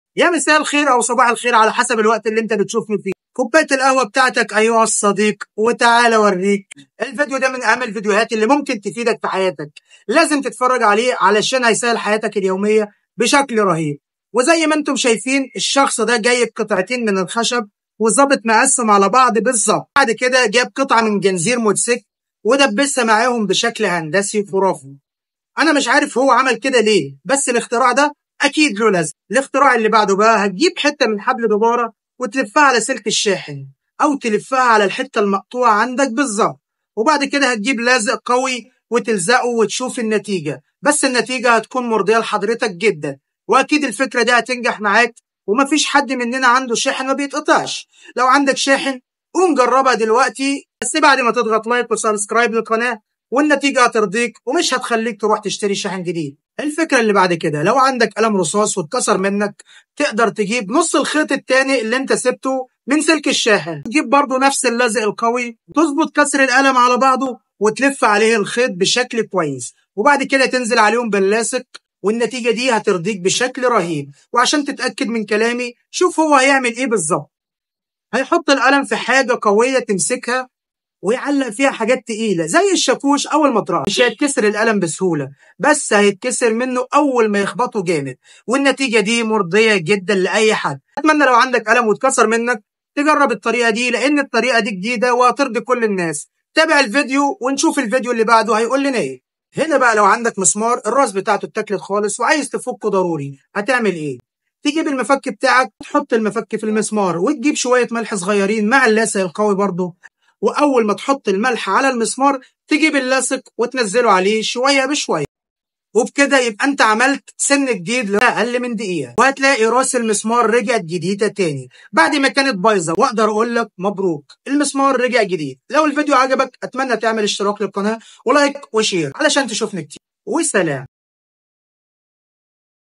افكار هتفيدك في حياتك بشكل رهيب 👌
افكار
فكره رائعه
فكره حلوه
افكار هتفيدك في حياتك
فكره مشروع
افكار مشاريع سهله
 مساء الخير على حسب الوقت اللي انت بتشوفني فيه الفيديو ده من اهم الفيديوهات اللي ممكن تفيدك في حياتك لازم تتفرج عليه عشان هيسهل حياتك اليوميه بشكل رهيب وزي ما انتم شايفين هنا الشخص جاب قطعتين من الخشب وظبط مقاسهم قد بعض بالظبط وبعد كده جاب قطعه من جنزير موتوسيكل وادبسها معاهم بشكل هندسي هو انا مش عارف اعمل كده ليه بس اكيد الاختراع ده ليه لازمه الاختراع اللي بعده بقى حته من حبل دوباره وتلف على سلك الشاحن او تلفه على الحته المقطوعه عندك بالظبط وبعد كده هتجيب لاصق قوي وتلزقه هتشوف نتيجه مرضيه لحضرتك جدا واكيد الفكره دي هتنجح معاك وما فيش حد مننا عنده شاحن ما بيتقطعش لو عندك شاحن قوم جربها دلوقتي بس بعد ما تضغط لايك وسبسكرايب للقناه والنتيجه هترضيك ومش هتخليك تروح تشتري شاحن جديد ثاني الفكره اللي بعد كده لو عندك قلم رصاص واتكسر منك تقدر تجيب نص الخيط الثاني اللي انت سبته من سلك الشاحن وتجيب برده نفس اللاصق القوي وتظبط كسر القلم على بعضه وتلف عليه الخيط بشكل كويس وبعد كده تنزل عليهم باللاصق القوي وبرده هتشوف نتيجه ترضيك بشكل رهيب وعشان تتاكد من كلامي شوف هو هيعمل لك ايه دلوقتي هيحط القلم في حاجه قويه تمسكها ويعلق فيها اشياء تقيله زي شاكوش اول مطرقه مش هيتكسر القلم هيتكسر منه اول ما يخبطه جامد بس والنتيجه دي مرضيه جدا لاي حد اتمنى لو حد عنده قلم واتكسر يجرب الطريقه دي لان الطريقه دي جديده وهترضي كل الناس تابع الفيديو ونشوف اللي بعده ايه
هنا لو عندك مسمار الروز بتاعته اتاكلت خالص وعايز تفكه ضروري هتعمل ايه تجيب المفك بتاعك تتفرج على المشهد ده هتحط المفك في المسمار وتجيب شويه ملح صغيرين مع اللاصق القوي برده انا مش عارف ليه اللاصق معانا في كل الفيديوهات واول ما تحط الملح على المسمار تجيب اللاصق وتنزله عليه وتسيبه اقل من دقيقه وهينشف ويعمل لك مكان للمفك جديد معنى كده ان راس المسمار رجعت جديده ثاني بعد ما كانت بايظه وهنا تقدر تفك براحتك وهنا اقدر اقولك الف مبروك المسمار رجع جديد تاني لو الفيديو عجبك اتمنى  تعمل اشتراك للقناه ولايك وشير عشان تشوفني كثير وسلام